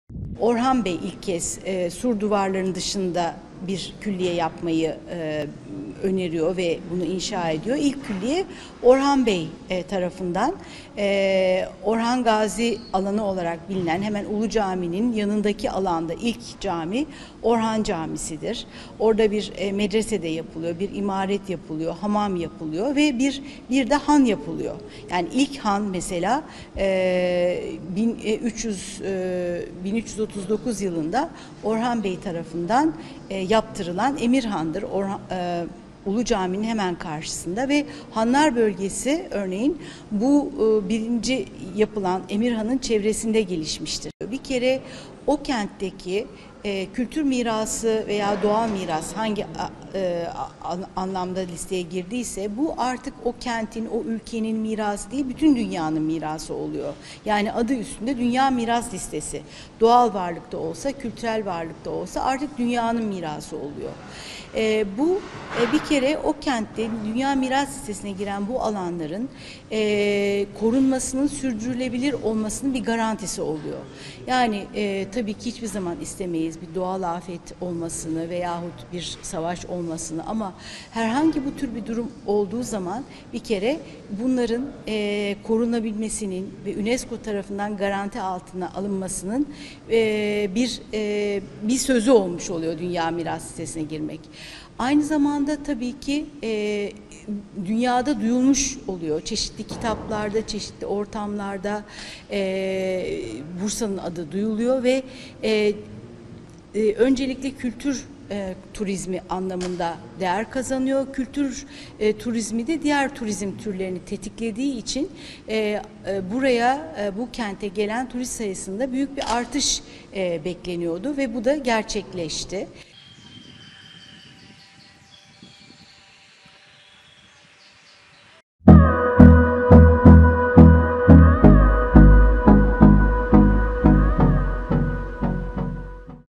Osmanlı Sultan Külliyeleri UNESCO İle Dünyada Yankı Uyandırıcak
Birleşmiş Milletler Eğitim, UNESCO Dünya Mirası Listesi ne 2014 yılında alınan Orhangazi, Hüdavendigar, Yıldırım, Yeşil ve Muradiye külliye leri dünya nın dört bir yanından ziyaretçi yi ağırlıyor.